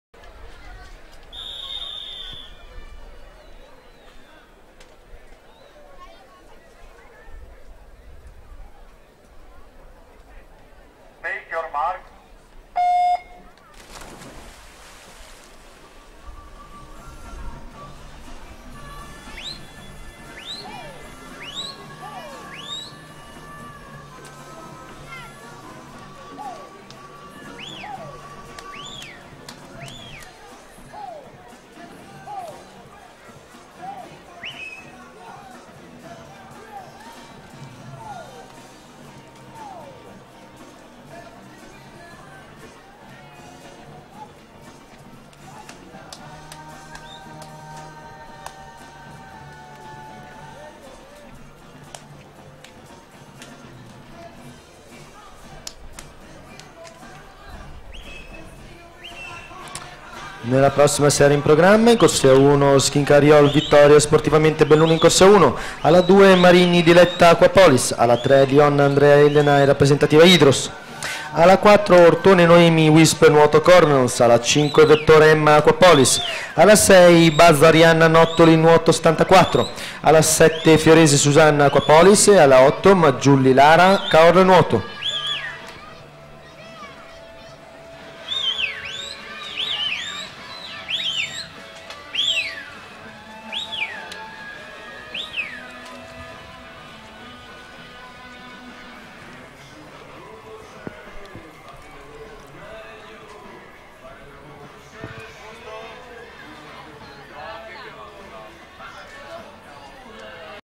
100 Rana Ragazzi Femminile (Serie 4) - 3 Treviso Swim Cup
Atleti partecipanti:
in corsia 1 CASETTO EMMA (2004) della soc. ADRIA NUOTO iscritta con 01:31.20
in corsia 2 PESCE GIULIANA (2004) della soc. AQUAPOLIS iscritta con 01:29.23
in corsia 3 SIMONETTO EMMA (2004) della soc. Rappr. GIS iscritta con 01:28.99
in corsia 4 TONDAT DAISY (2003) della soc. UISP NUOTO CORDENONS iscritta con 01:28.23
in corsia 5 SVALDI EMMA (2004) della soc. ANTARES NUOTO iscritta con 01:28.54
in corsia 6 MONTAGNER ALICE (2003) della soc. PIAVE NUOTO iscritta con 01:29.13
in corsia 7 MARTINOLLI ELENA (2004) della soc. CENTRO SPORTIVO HOF iscritta con 01:30.00
in corsia 8 BEVACQUA PANIGAI ELENA (2003) della soc. STELLA MARINA iscritta con 01:31.42